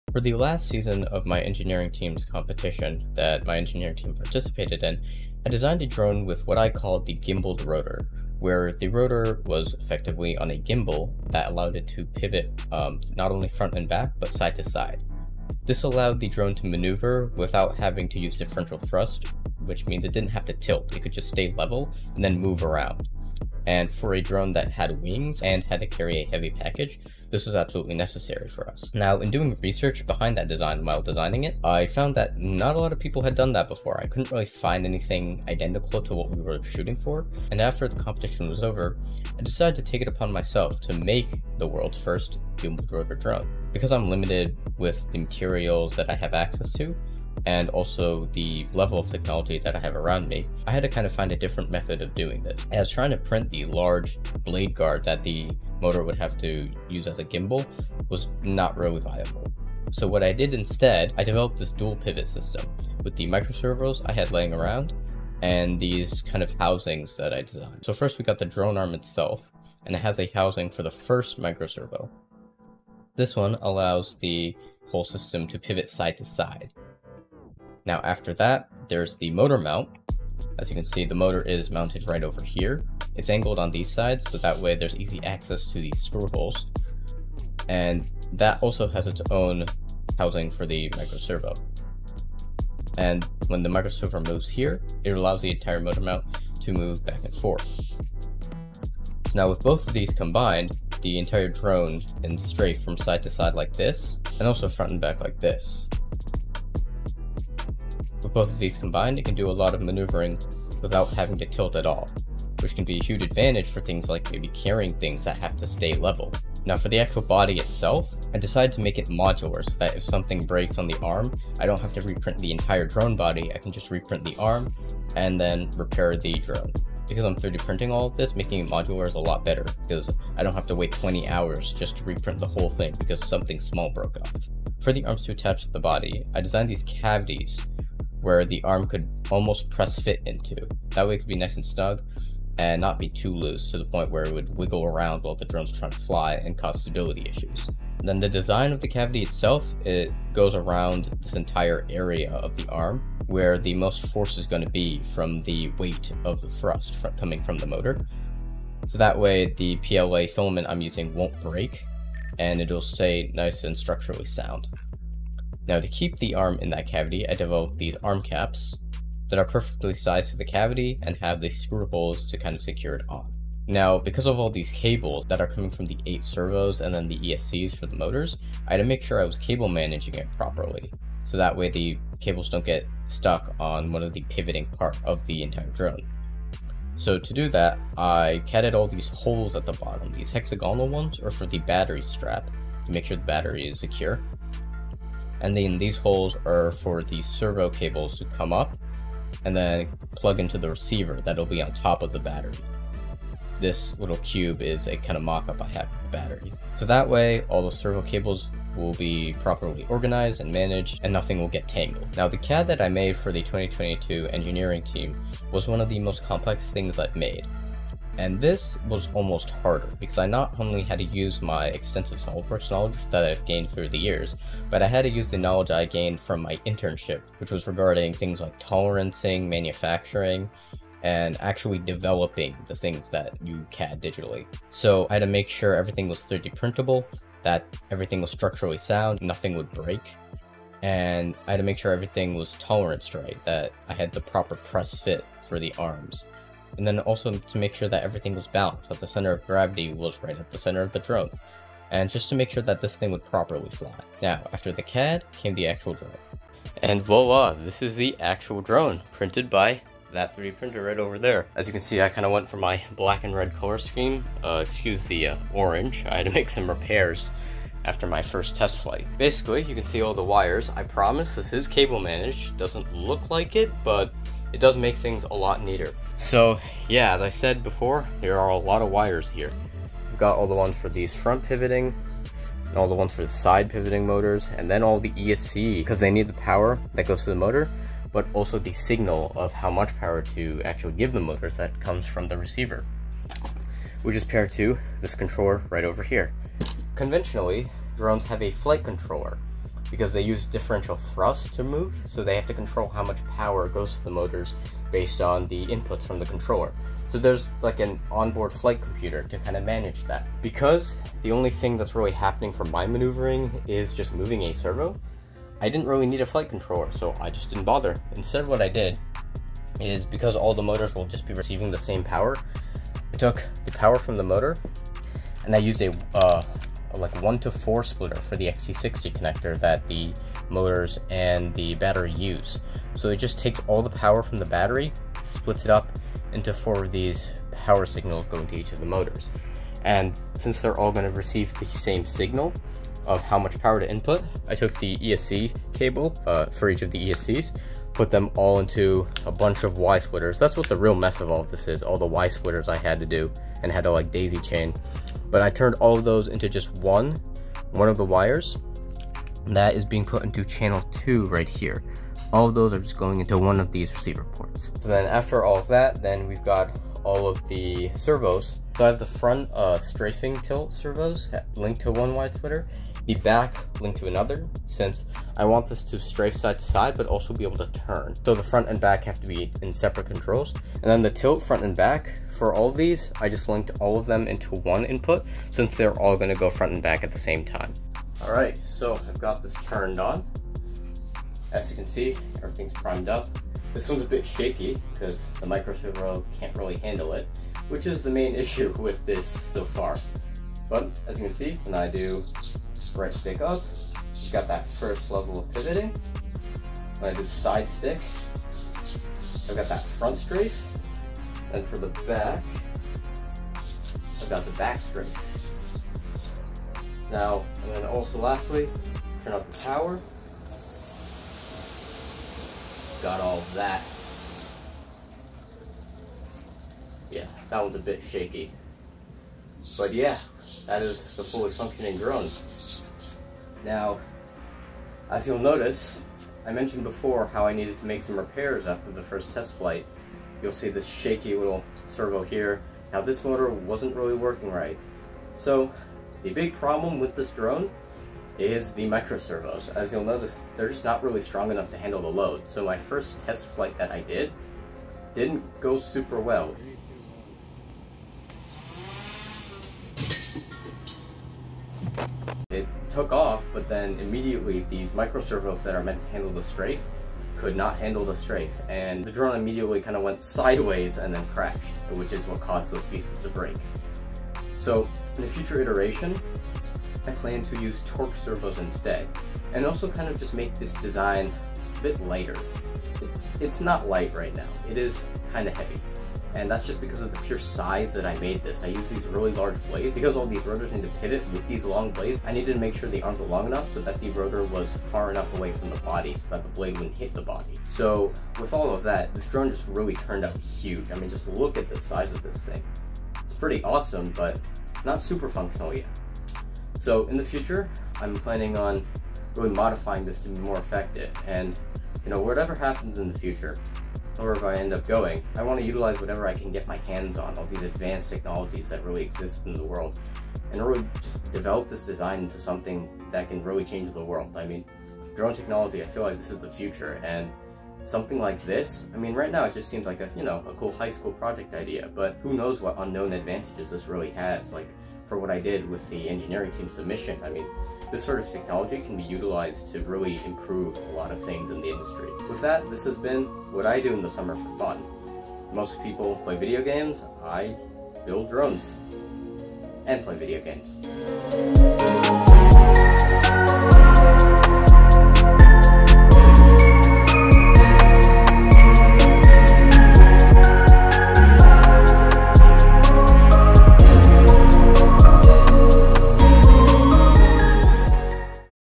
The Gimballed Rotor Drone: Can it be done? Well I certainly tried - PART 1
I made this over the summer for college app purposes, and now that that's all over, I figured I'd document the whole project through videos uploaded on YouTube.